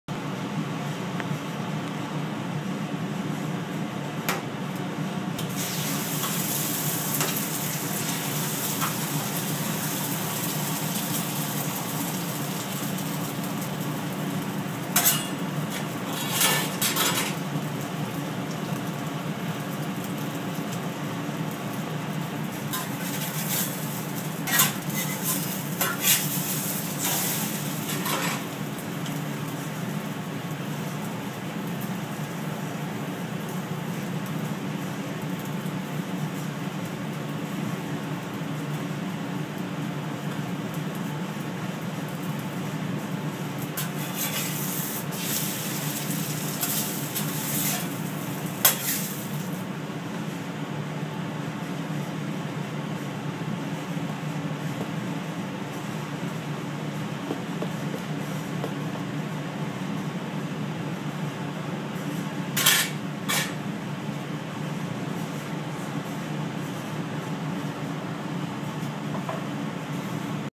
Chef David J Alvarez (Over easy egg in ONE minute)
How to cook an egg over egg in just minutes on a flat top grill